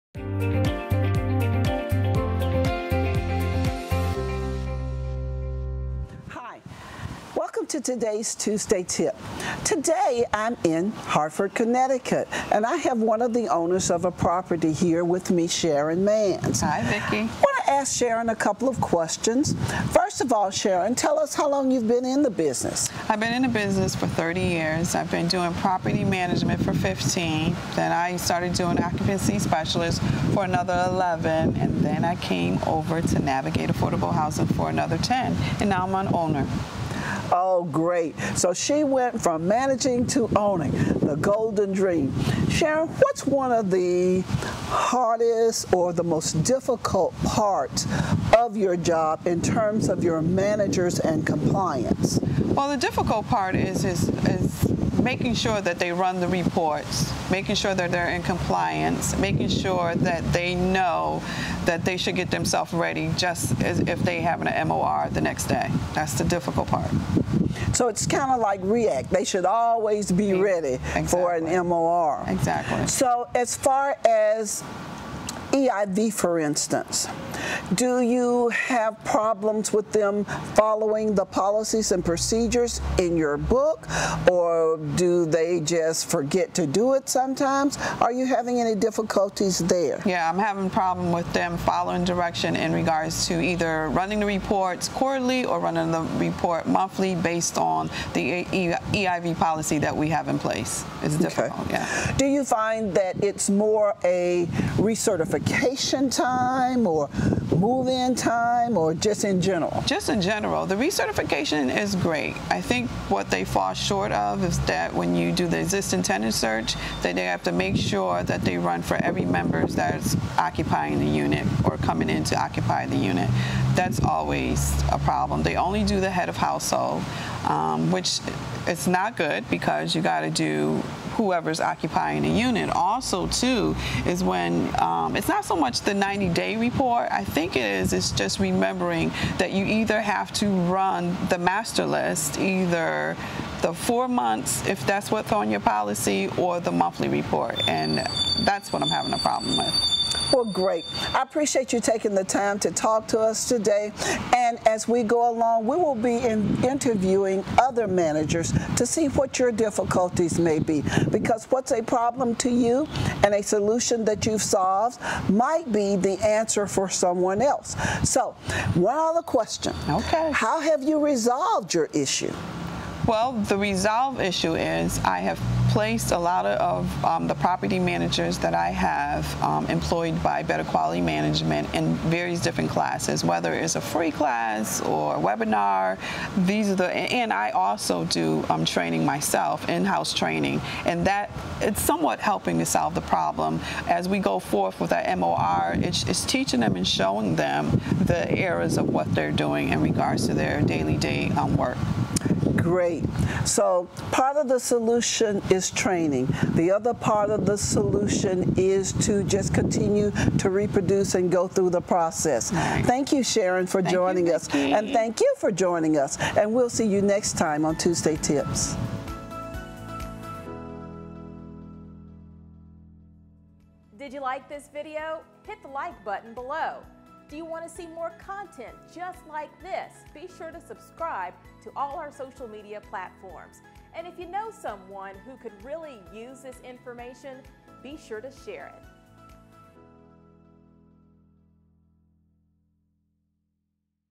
Tuesday Tip in Connecticut! Vickie talks with Sharon Manns
Vickie talks with Sharon Manns, a property owner in Connecticut!